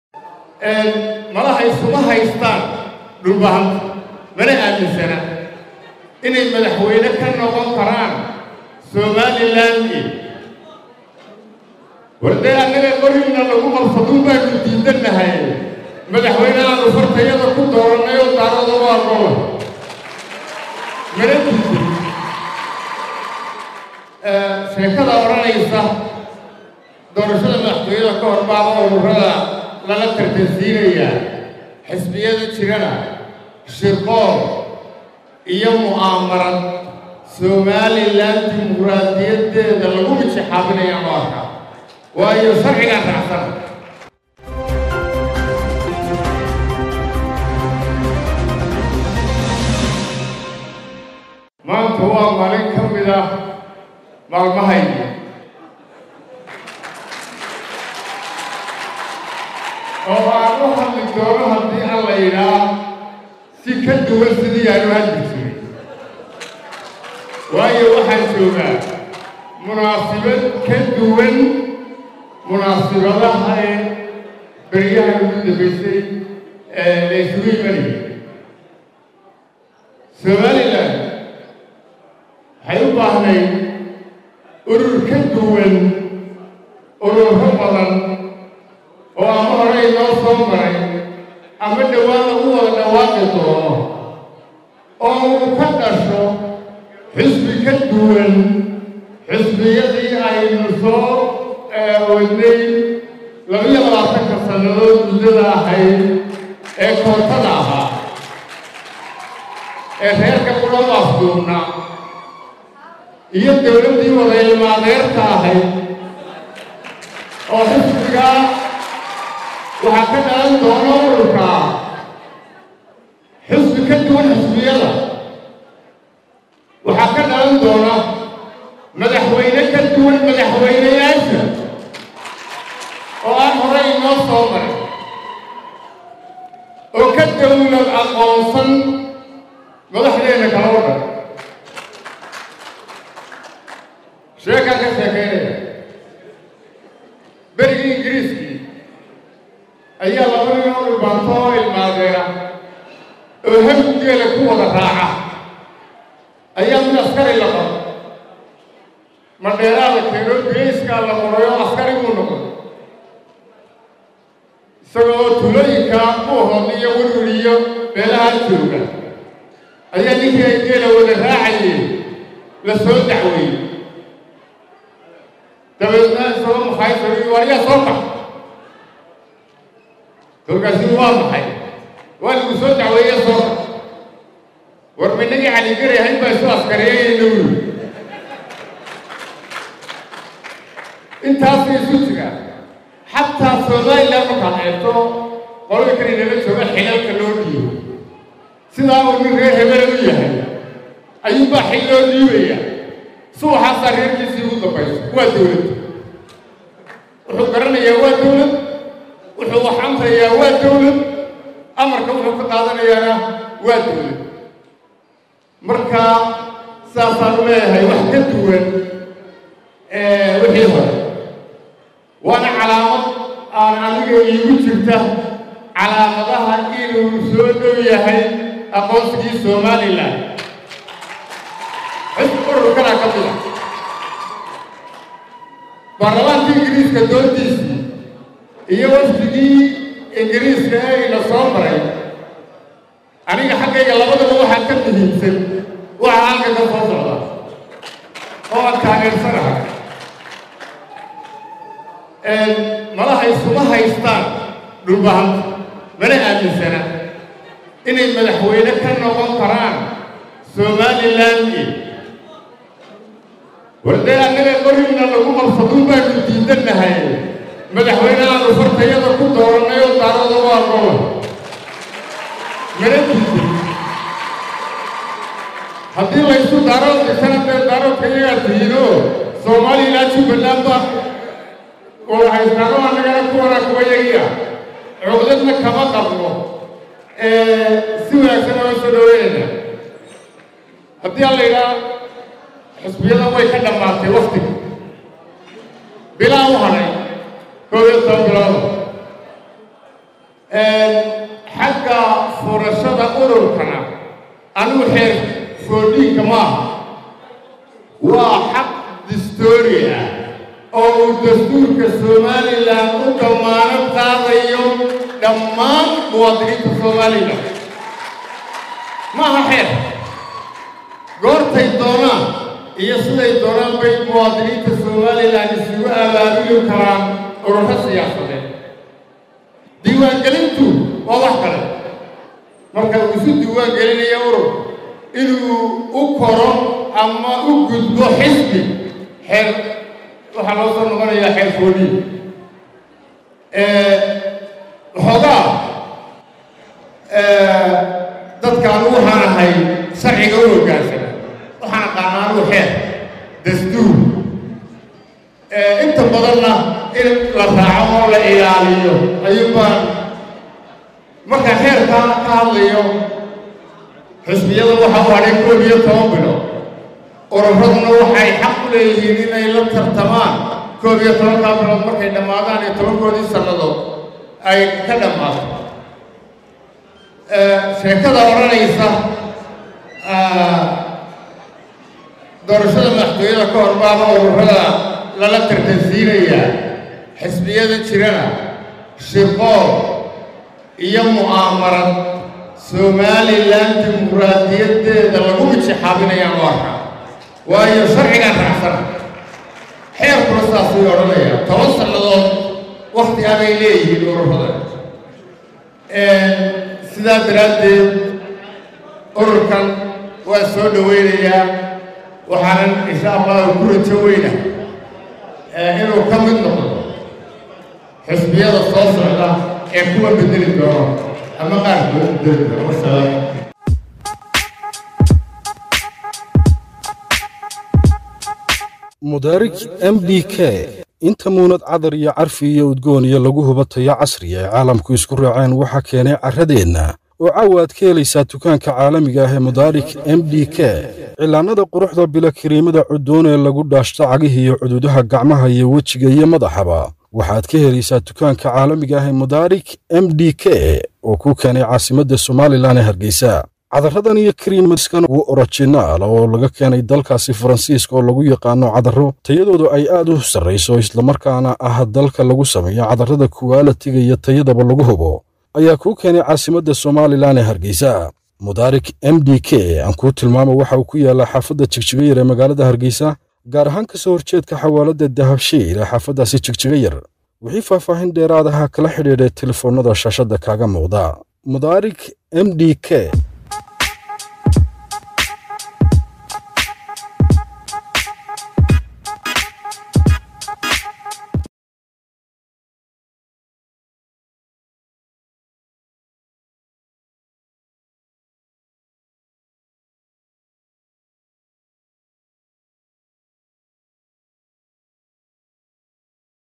"Anigu Shir-qool baan u arkaa lagu mijo-xaabinaayo Dimoquraadiyada.." Siyaasi Cali-guray
Siyaasi Cali-guray oo ka dayriyay ururada lala tartansiinayo xisbiyadii Jiray == DHAMAYS MEDIA PRO==